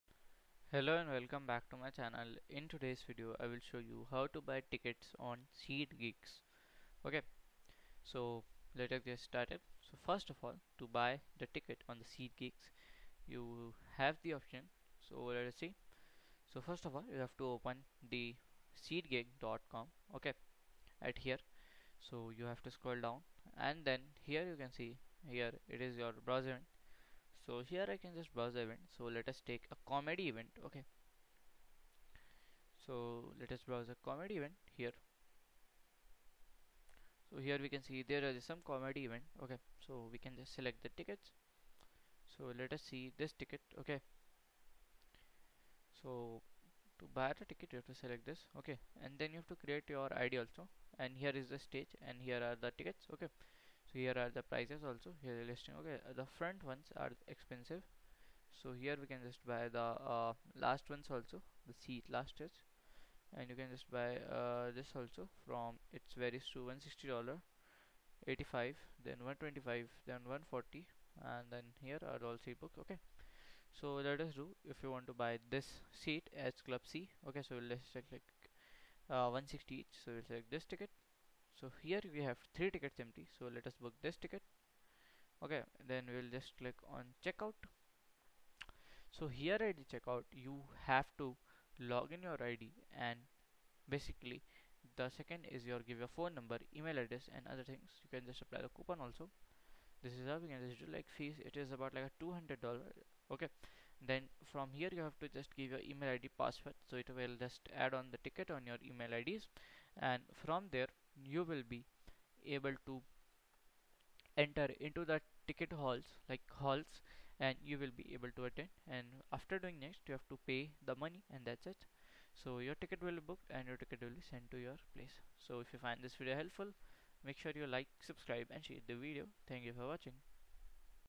✅ How To Buy Tickets on SeatGeek (Full Guide)
Discover the art of purchasing tickets with our comprehensive guide on buying tickets on SeatGeek! 🎟️💳 Learn the step-by-step process to find, purchase, and secure tickets for your favorite events, ensuring a seamless and enjoyable ticket-buying experience. Watch this tutorial for expert tips and make your ticket purchases on SeatGeek hassle-free.

Key Learning Points:
 • Step-by-step guide on buying tickets on SeatGeek
 • Find, purchase, and secure tickets for a seamless ticket-buying experience
 • Make your ticket purchases hassle-free with this complete guide.